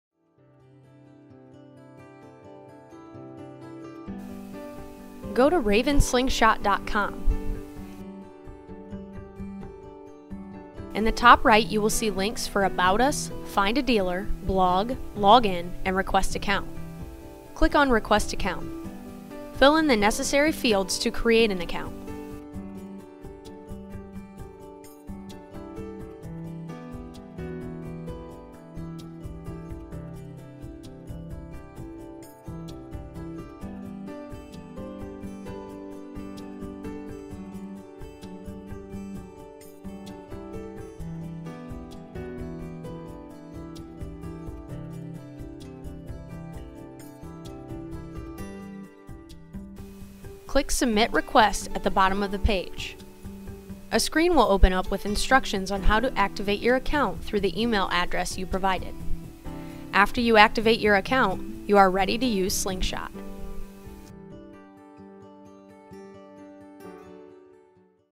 UHarvest™ Slingshot® Creating A New Account
Take full advantage of your UHarvest Data Management System by creating an account with Slingshot to manage all of your reports. today!